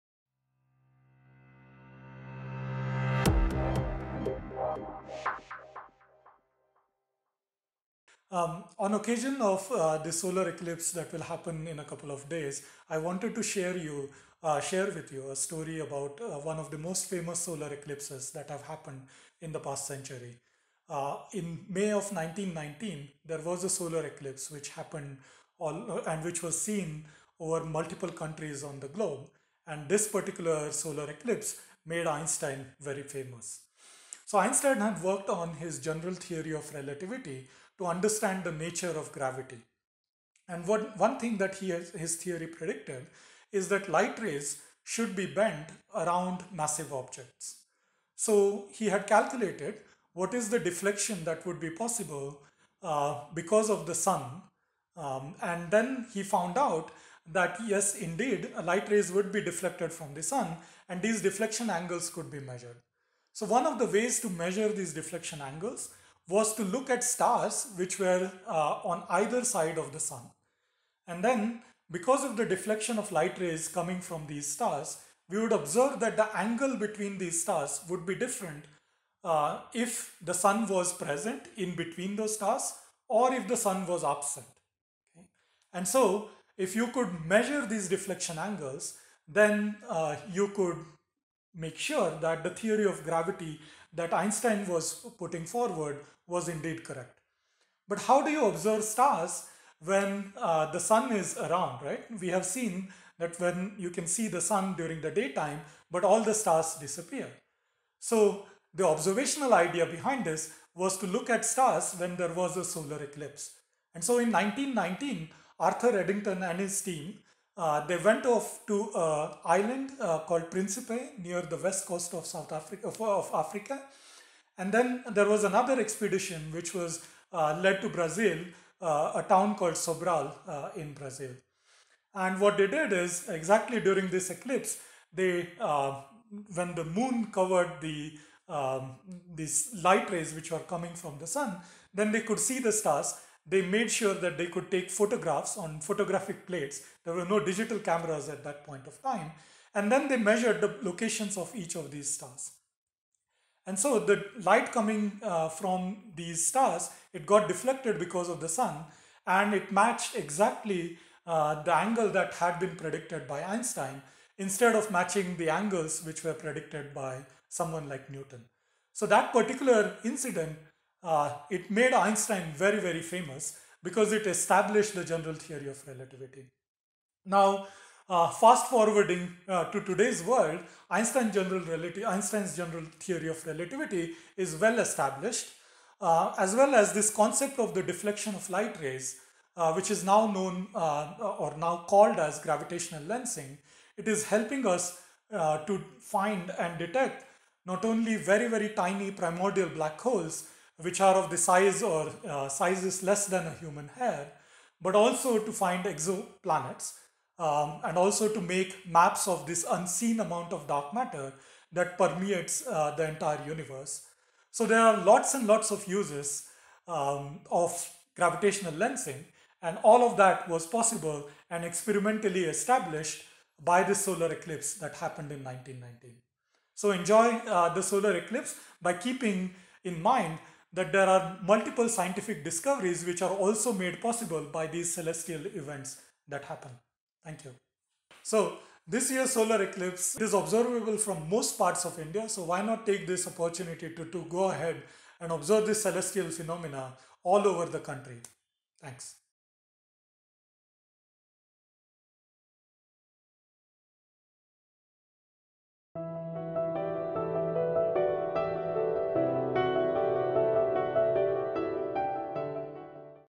The famous Solar Eclipse of 1919 that made Einstein Famous
Prof Surhud More, Associate Professor at IUCAA talks about the 1919 Solar eclipse which gave us groundbreaking evidence of relativity by observing the bending of the light.